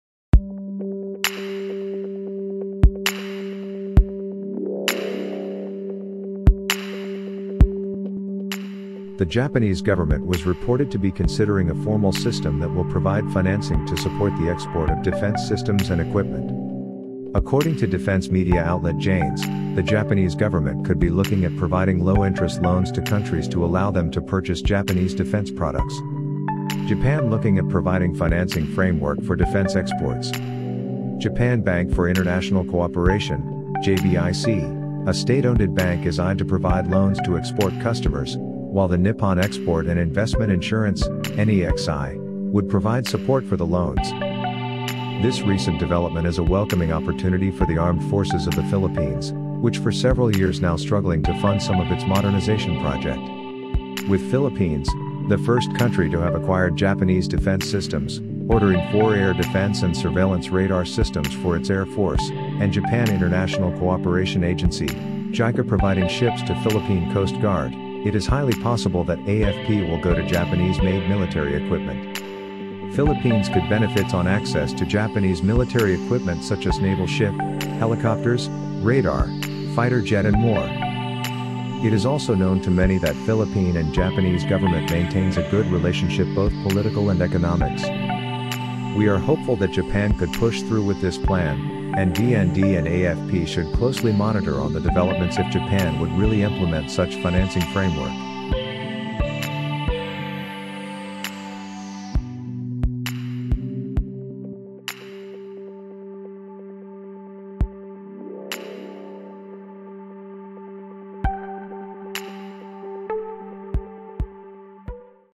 AFP Will Benefit on Japan Plan to Finance Defense Exports
The Japanese Government was reported to be considering a formal system that will provide financing to support the export of defense systems and equipment. According to defense media outlet Jane’s, the Japanese Government could be looking at providing low-interest loans to countries to allow them to purchase Japanese defense products. Japan looking at providing financing framework for defense exports. Japan Bank for International Cooperation (JBIC) a state-ownded Bank is eyed to provide loans to export customers, while the Nippon Export and Investment Insurance (NEXI) would provide support for the loans.

This recent development is a welcoming opportunity for the Armed Forces of the Philippines, which for several years now struggling to fund some of its modernization project. With Philippines, the first country to have acquired Japanese defense systems, ordering 4 air defense and surveillance radar systems for its air force, and Japan International Cooperation Agency - JICA  providing ships to Philippine Coast Guard, it is highly possible that AFP will go to Japanese-made Military Equipment. Philippines could benefits on access to Japanese military equipment such us Naval Ship, Helicopters, Radar, Fighter Jet and more.